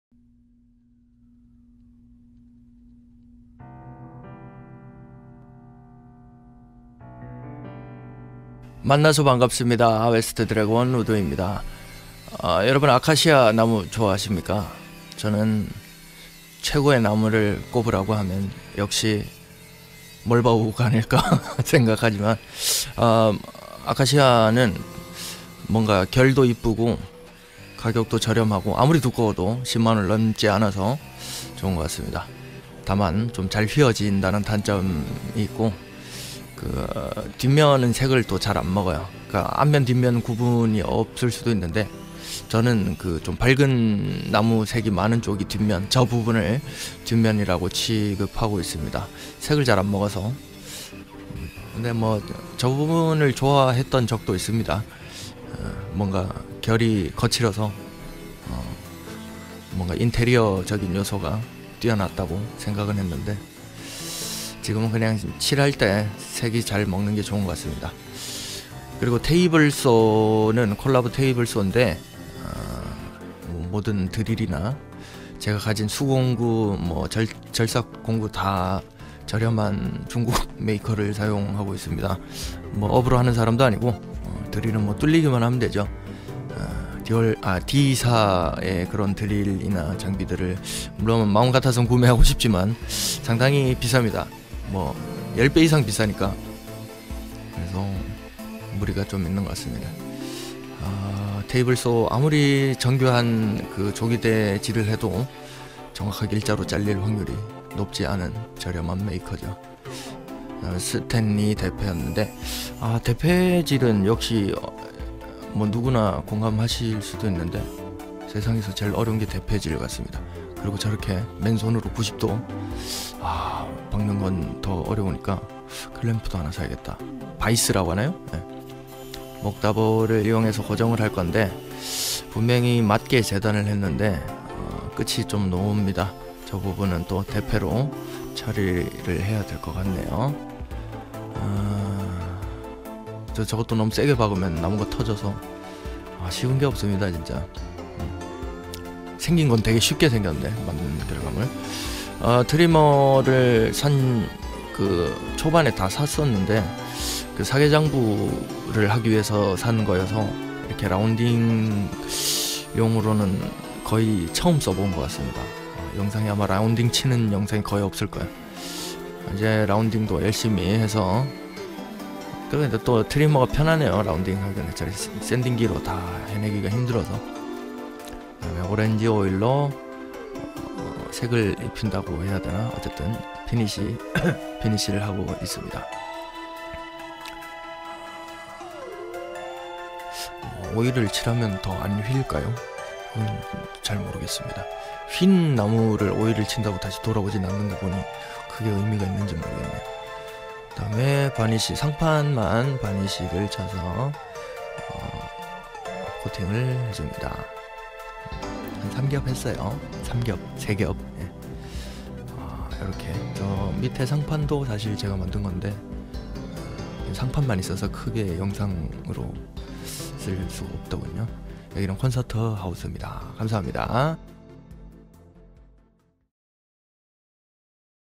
원목 모니터스탠드 만드는 방법! Monitor Stand DIY #40
안용~? 이름이 이서용이라
웨스트드래곤우드임 ㅋㅋ
목공소를 창업하기전
연습하는 공간에서 찍는 영상임..
시청감사!

ㅡㅡㅡㅡㅡㅡㅡㅡㅡㅡㅡㅡㅡㅡㅡㅡㅡㅡㅡㅡㅡㅡ
사용장비
슬라이딩 레이져 각도절단기

ㅡㅡㅡㅡㅡㅡㅡㅡㅡㅡㅡㅡㅡㅡㅡㅡㅡㅡㅡㅡㅡㅡㅡ
목재주문